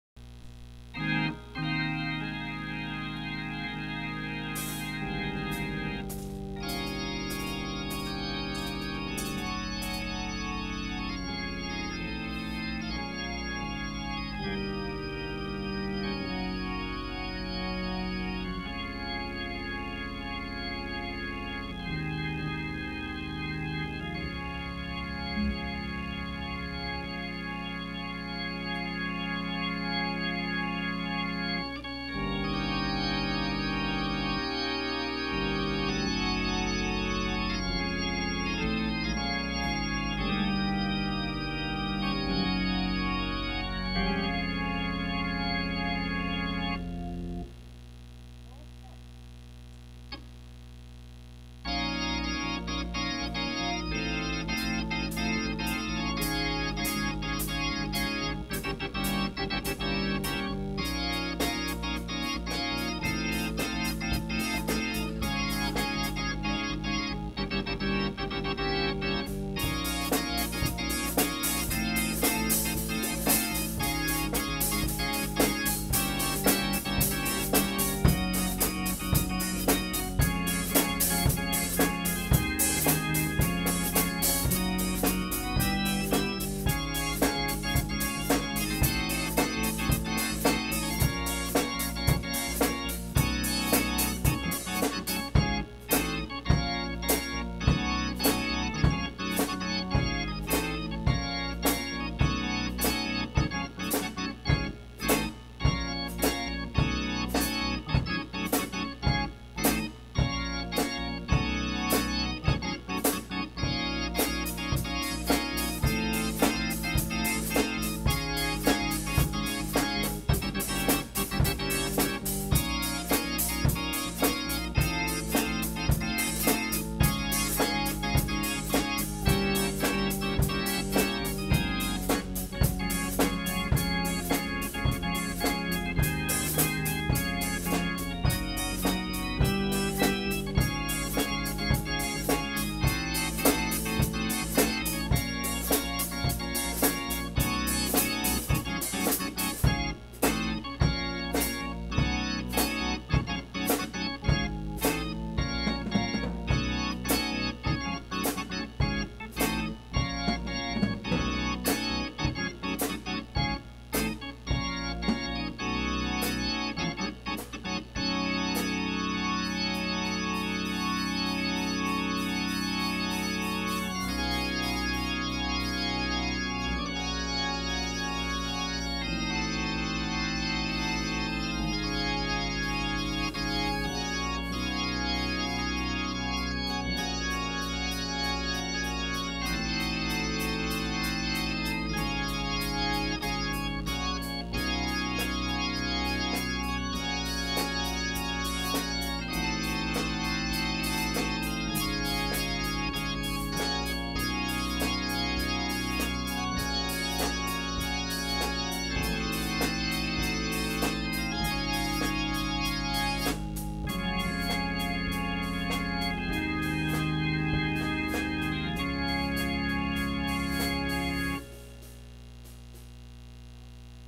O' Talented: FEMALE DRUMMER: On the keys!!!
I'm just playin some song I made up. Nothing major. I'm not all that so don't be tryna go sick on me!!! I'm just learning how to play.